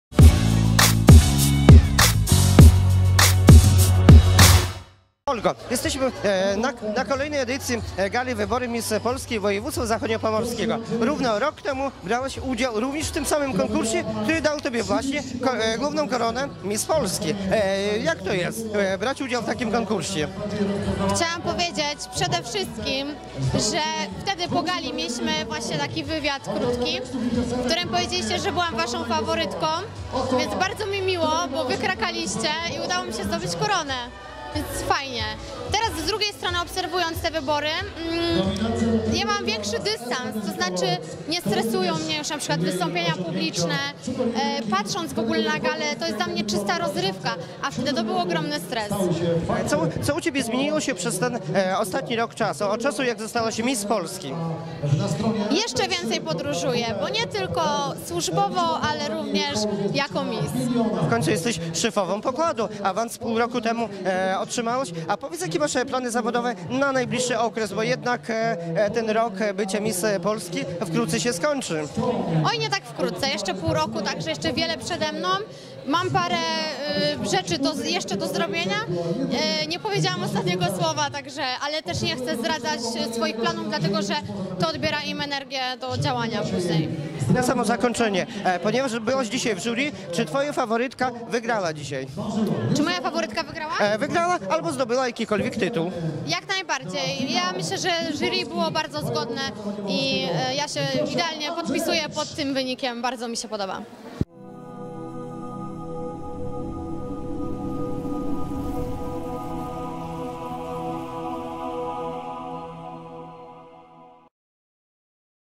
Olga Buława: "Moja kandydatka wygrała konkurs Miss Polski Woj. Zachodniopomorskiego 2019"
Olga Buława: "Moja kandydatka wygrała konkurs Miss Polski Woj. Zachodniopomorskiego 2019", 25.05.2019

Reporter: Andrzej Tarkowski-Kiliszewski
Operator kamery: Łukasz Kostrzewski
Montaż: Andrzej Tarkowski-Kiliszewski
Produkcja: Telewizja ATV sp.z o.o. 2019